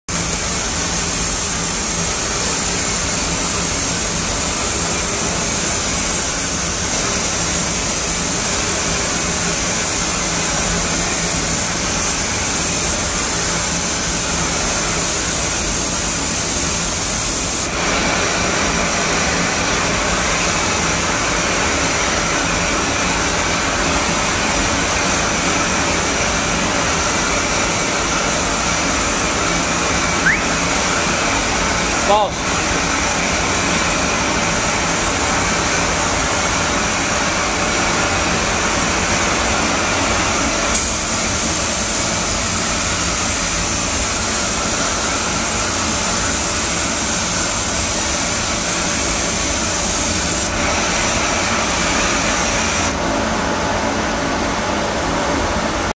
Chesterfield College Spray Booth 5
Chesterfield College Auto Body & Paint learners working in spray booth